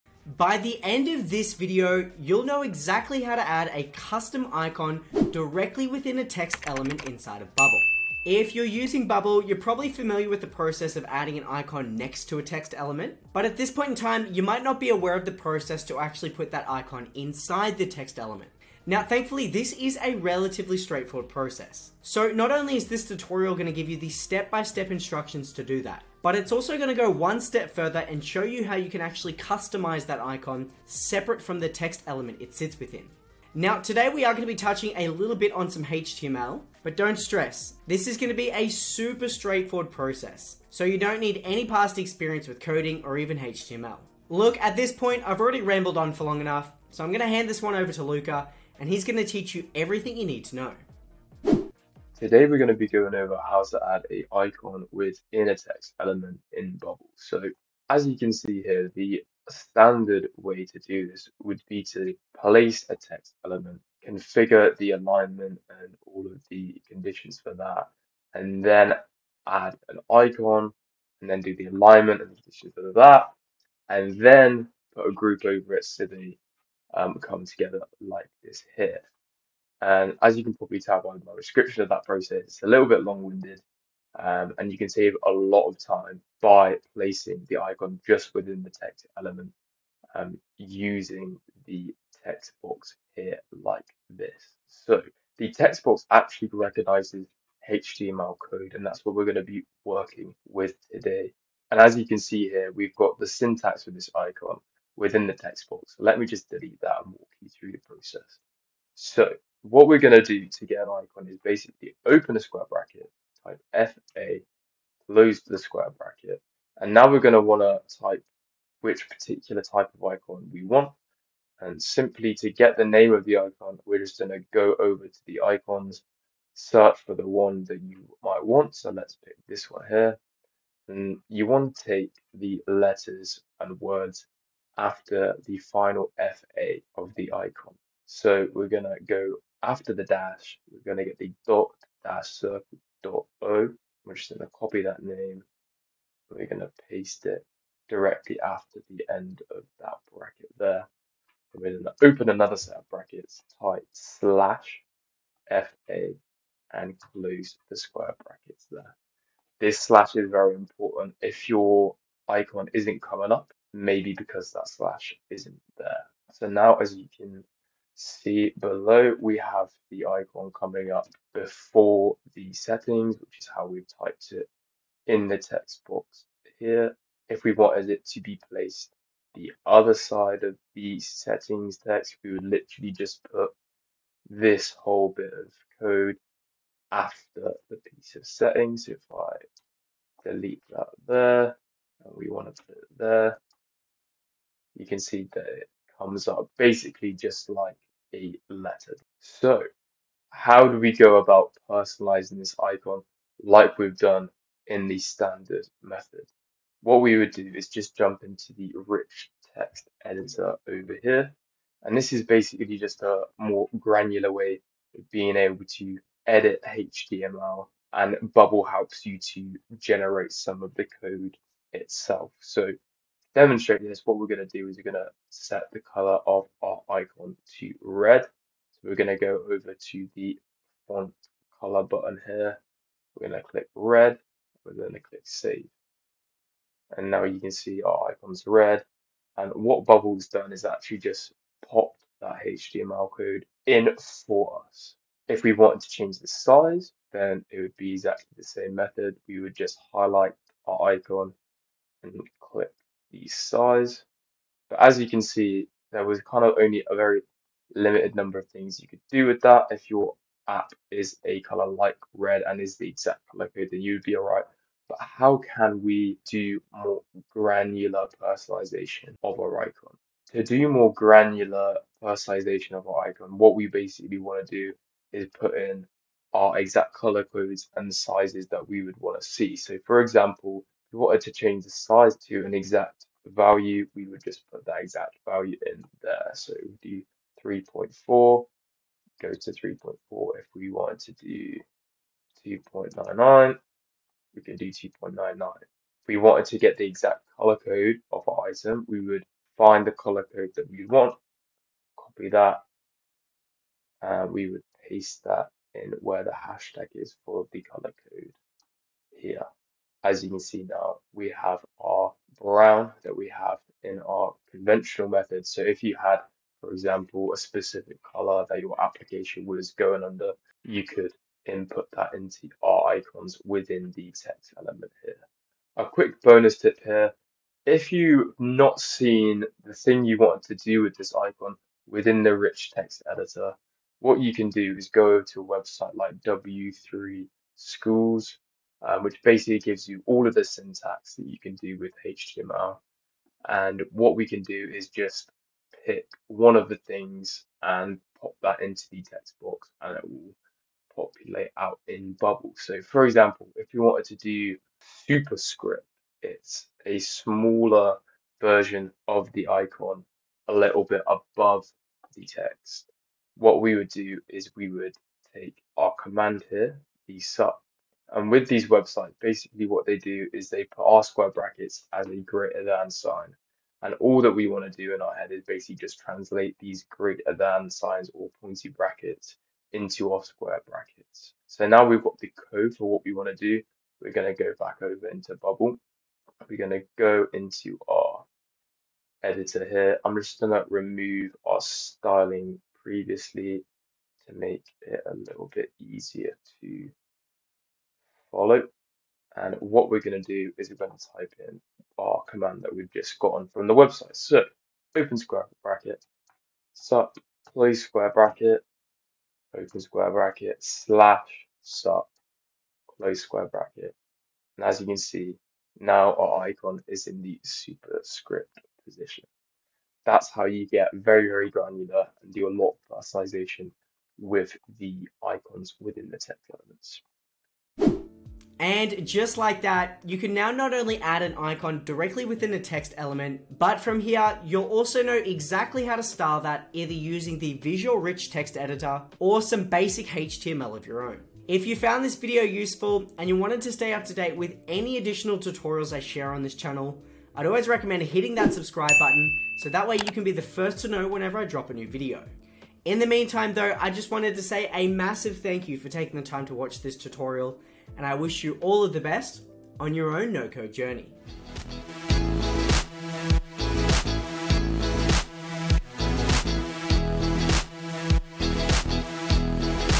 How To Add An Icon In A Text Field - Bubble.io Tutorial
If you’re building an app within Bubble, you’ll know it’s possible to add both an icon element, alongside a text element inside a group. This process, however, can be tedious and unnecessary. As an alternative, placing the icon element inside of a text field is a much better solution.

In this video, you’ll learn exactly how to add an icon directly within a text element. The process includes:

1. Sourcing the name of your icon
2. Adding some basic HTML to display an icon within a text field
3. Changing the style of the icon using the visual rich text editor
4. Changing the style of the icon using custom HTML

🔗 Mentioned link 🔗

⏱ Timeline: ⏱
00:00 - Course overview
00:52 - Adding an icon within a text field
03:19 - Styling the icon using the rich text editor
05:26 - Styling the icon with custom HTML
07:08 - Additional insights